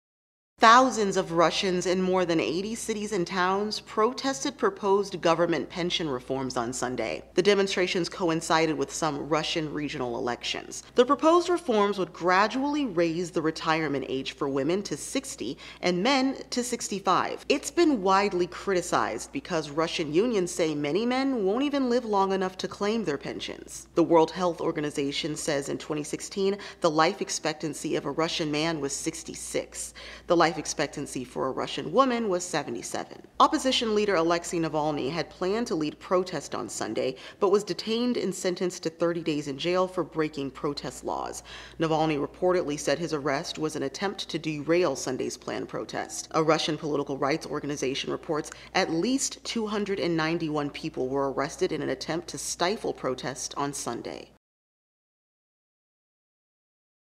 Thousands of Russians protest pension reform
The proposed reform would gradually raise women's retirement age to 60 and men's to 65.